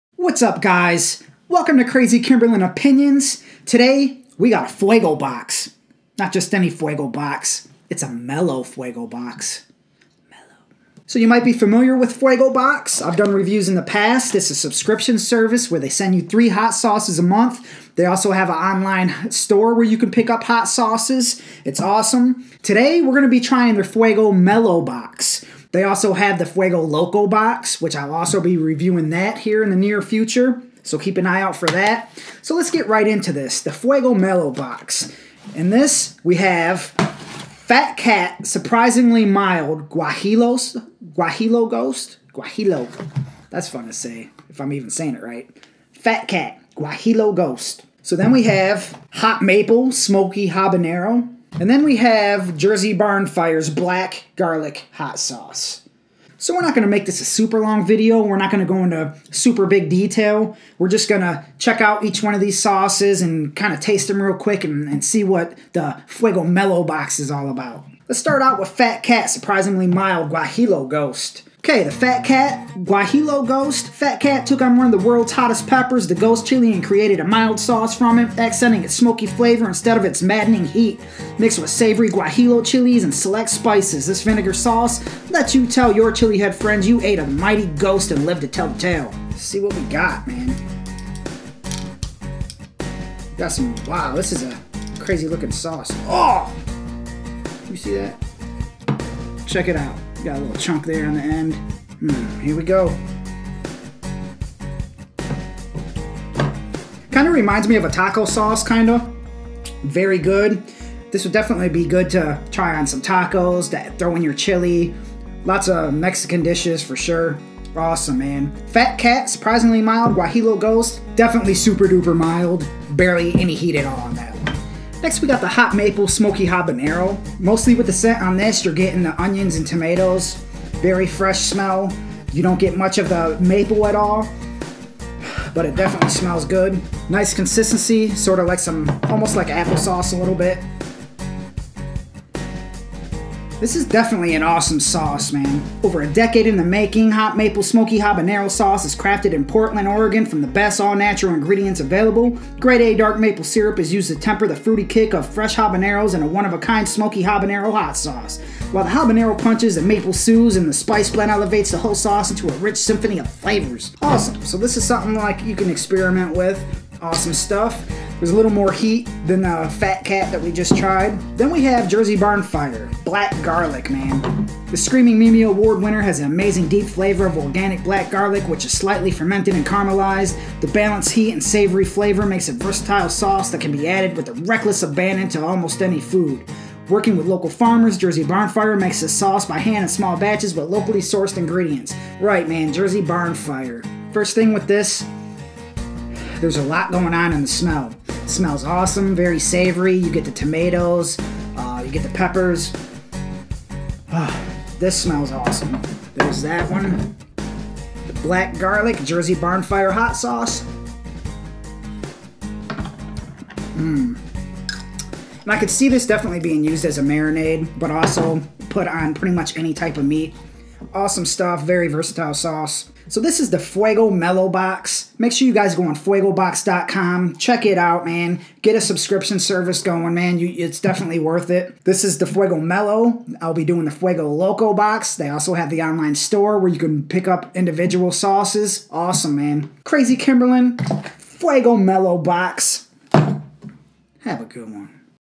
Fuego Box -Mellow- Subscription Review
Fuego Box is a subscription service where you get a box of Hot sauces each month.  They also have an online store where you can buy sauces individually. In this one we try Fuego box MELLOW edition!  Check them out here: